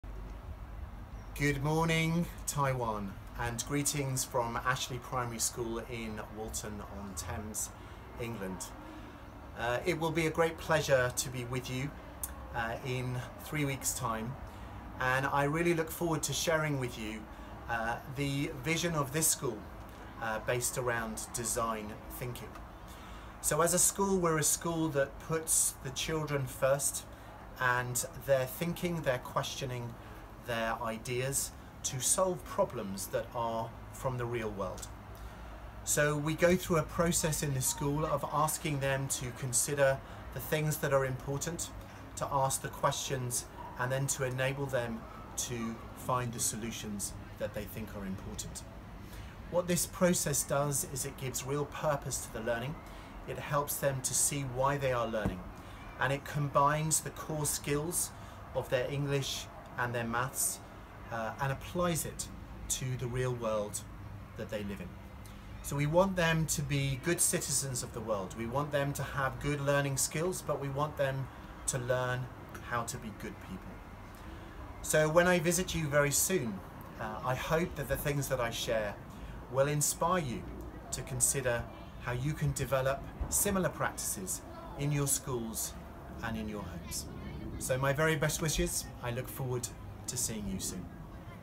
理查﹒鄧恩校長跟臺灣的教育者們問好，淺談其治校理念！
國際課程設計獎得主：理查・鄧恩校長邀請您一同來設計臺灣教育環境：
唯一臺灣場！由英國 DFC 的響應夥伴遠道而來與臺灣的教育工作者們分享，邀請關心教育與下一代的成長，期待接受一些國際教育理念交流與激盪的你。
7/26 （三）高雄場：講座＋工作坊
7/27 （四）臺中場：講座＋工作坊
7/29 （六）臺北場論壇：與葉丙成老師、陳清圳校長對談交流，聊聊教育環境的最佳情境！